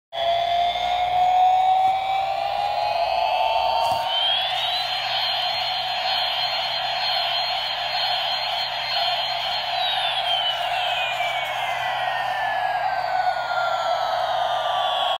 Вертолет на батарейках код 100432
вертолет ездит, крутится вокруг своей оси под звуковое сопровождение